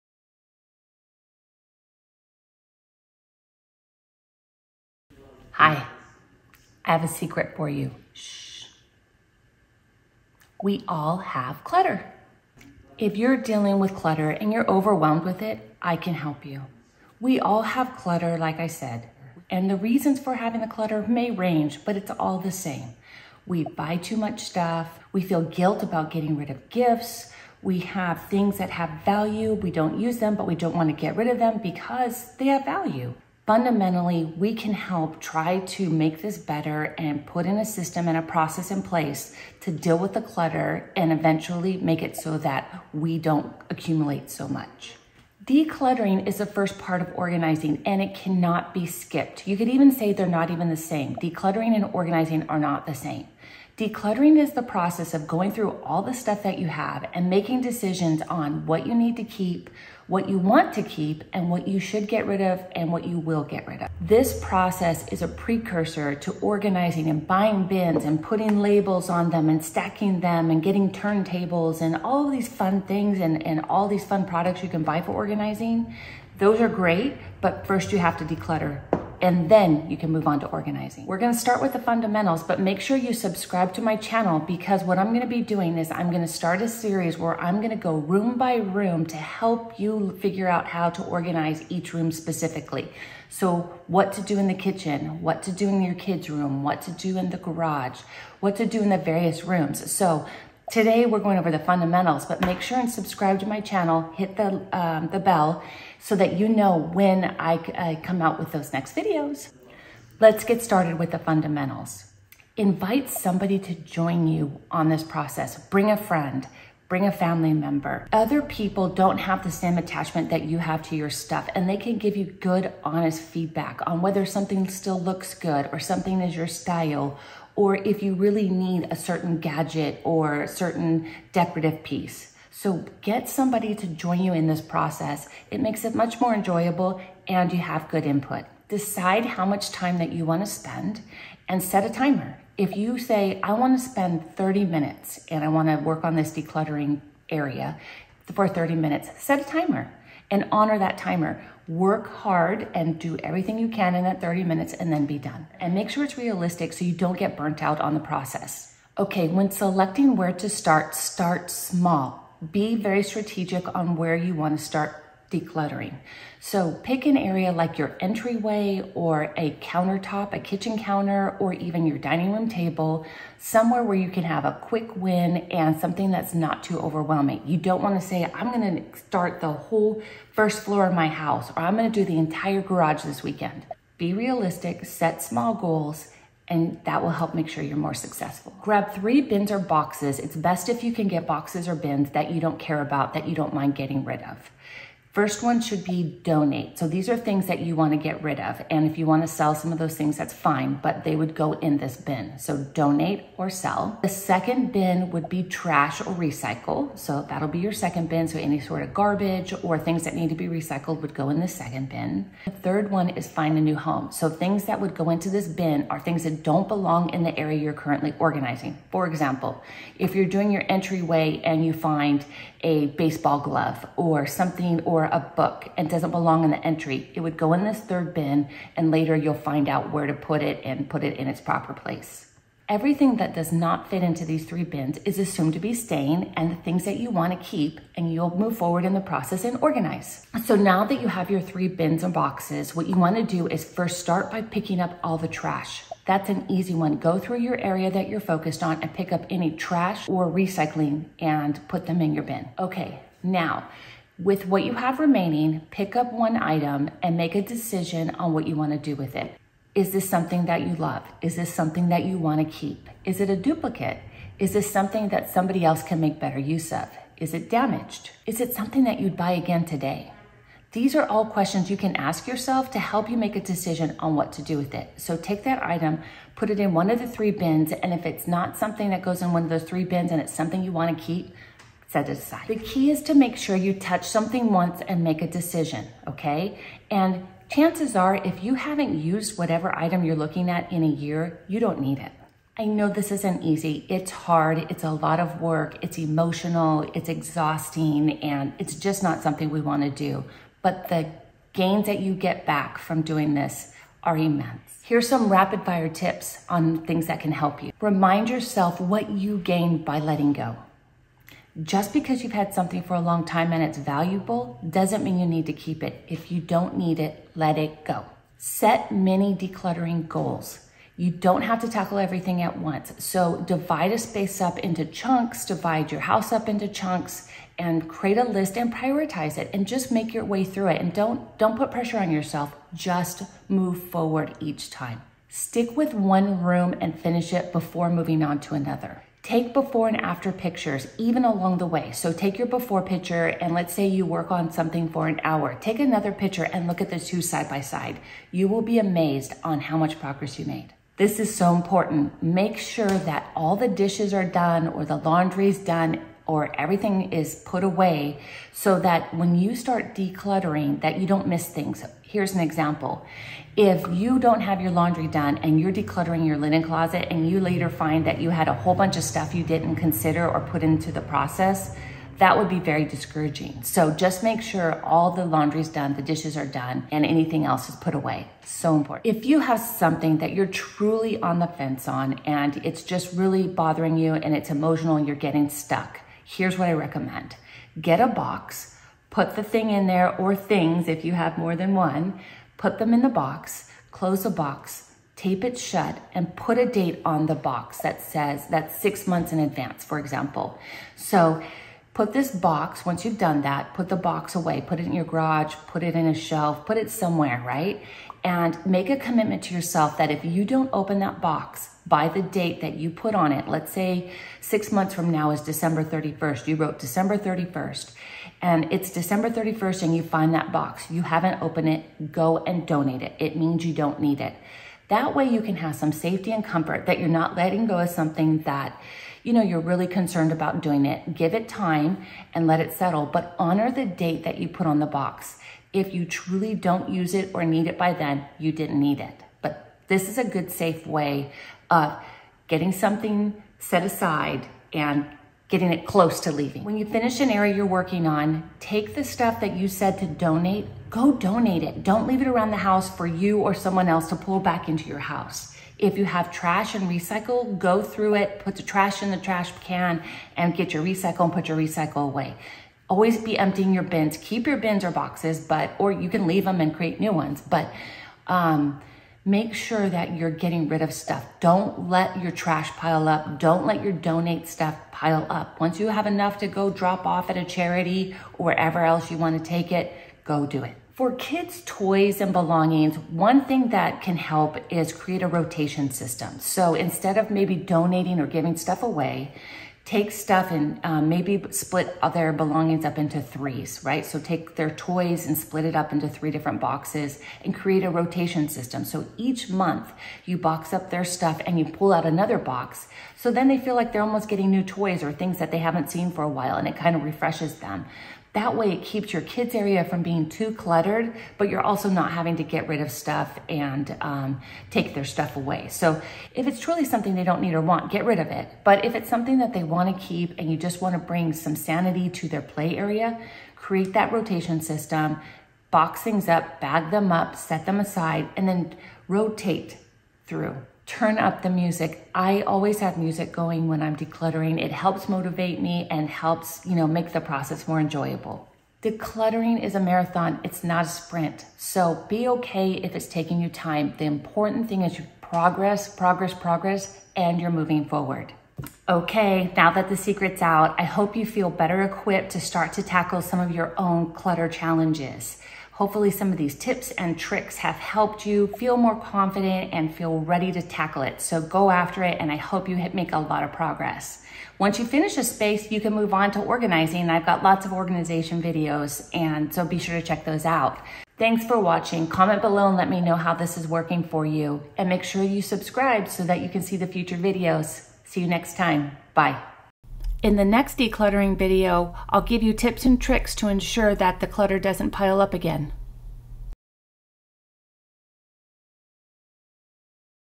Quick tips to organize and declutter
This video includes practical steps on how to approach and start your next decluttering project.  This is the first video of a decluttering series, upcoming videos will detail how to approach decluttering in different parts of the home, ie kitchen, closet, bedroom, garage, etc.  Also, we will address how to make sure the clutter doesn't start to build up again.

Please let me know what you think of this video in the comments!